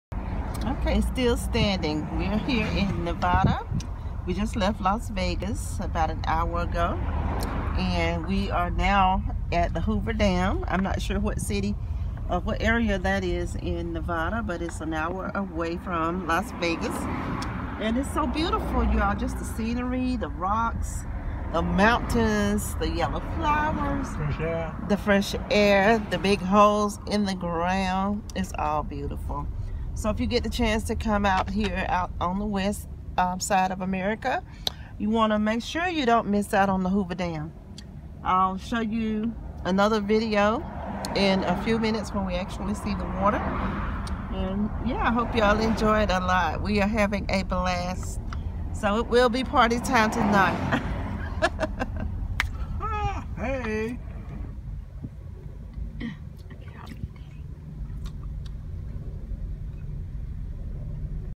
HOOVER DAM!!!! - on way to Arizona (from LasVegas Nevada)
#34 On the way to the HooverDam in LasVegas Nevada!